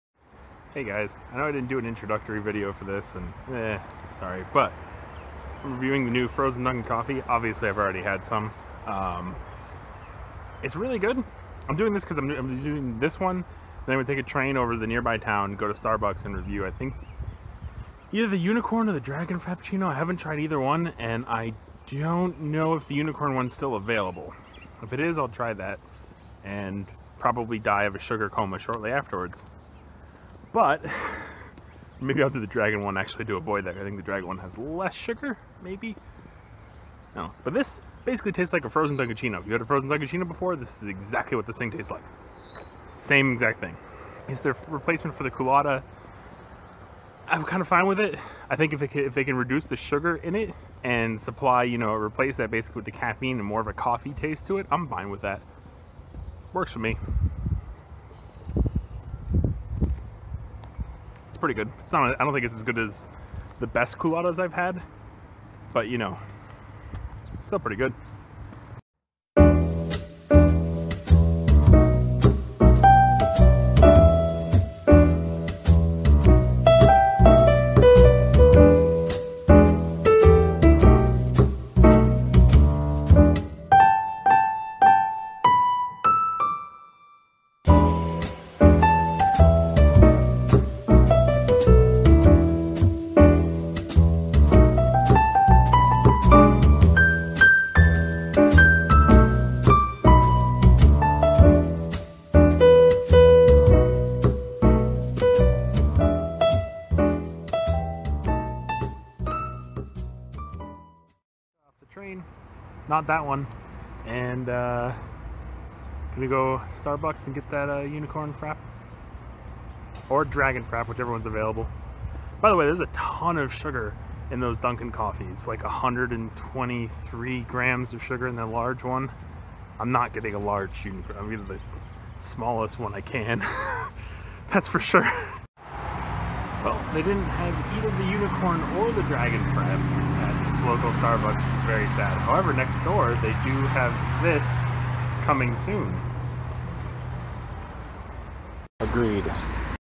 Are Unicorns Real?
We set out on a quest to try the Unicorn Frappe and Frozen Dunkin Coffee.

Note: This is not a paid ad from either Dunkin Donuts or Starbucks.